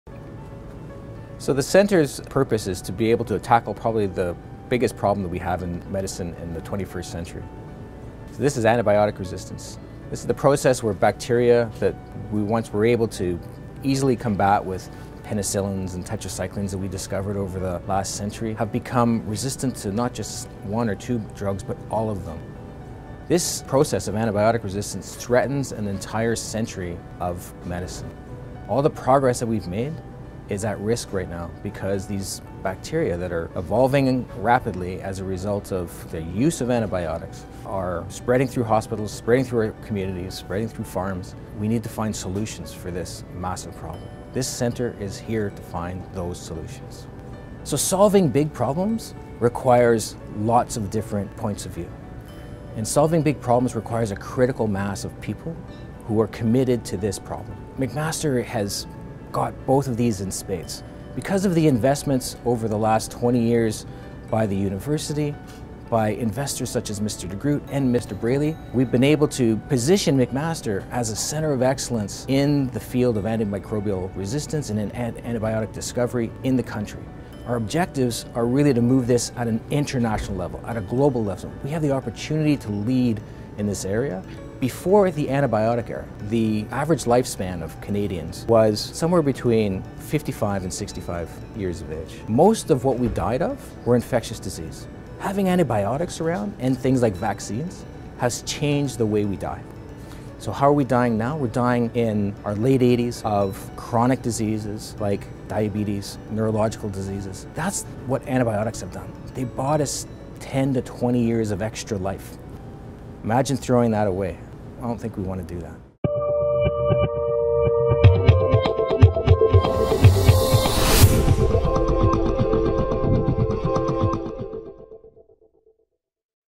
David Braley Centre for Antibiotic Discovery
The new David Braley Centre for Antibiotic Discovery will operate from the Michael G. DeGroote Institute for Infectious Disease Research at McMaster University. The research centre will be dedicated to tackling the growing global threat of antimicrobial resistance.